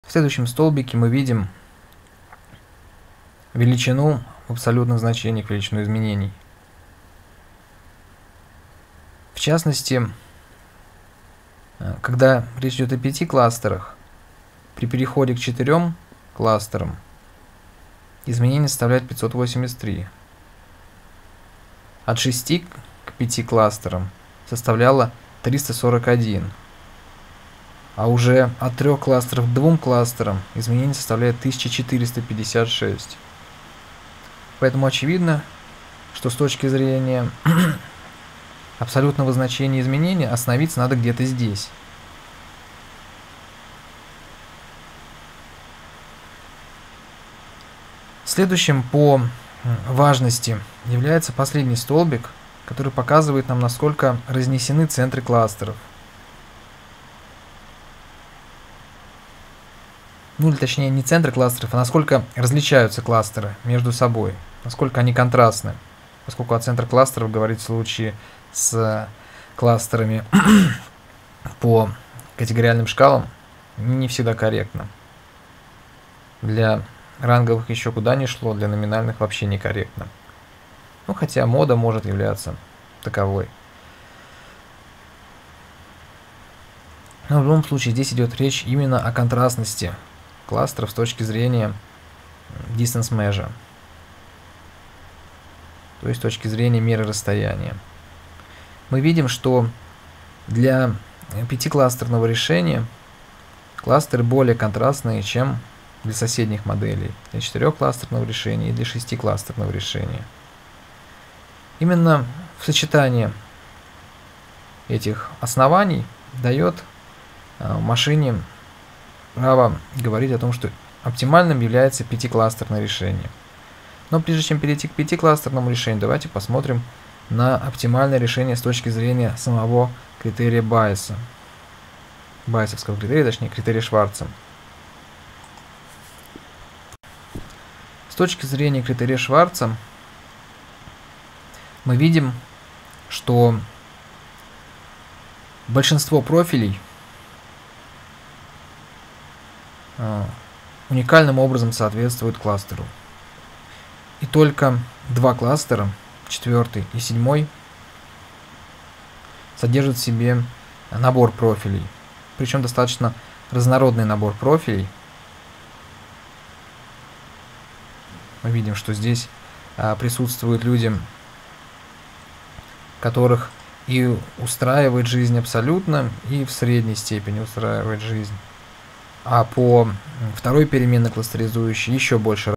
Двухступенчатый кластерный анализ 6 Категориальные переменные
Рассмотрим абсолютную величину изменения критерия Шварца -- здесь важен наибольший скачок; он указывает на вторую из оптимальных моделей. Величина контрастности кластеров (Ratio of Distance Mesures) -- чем она выше, тем лучше. Она указывает на третью из оптимальных моделей, которую и выбрал компьютер.
Навигация по фрагментам видео (после перехода используйте область навигации по странице справа).